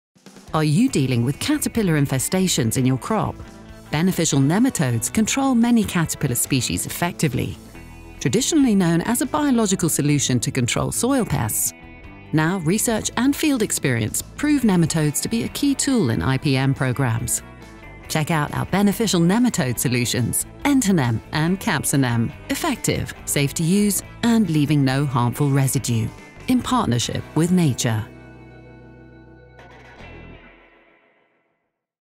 Effective caterpillar control in your greenhouse
Dealing with caterpillar infestations in your crop? Discover how beneficial nematodes can effectively control many caterpillar species. Watch this video to learn how nematodes have become a key tool in Integrated Pest Management (IPM) programs. Introducing Entonem and Capsanem, our safe and effective beneficial nematode solutions that leave no harmful residue. Partner with nature to protect your crops today!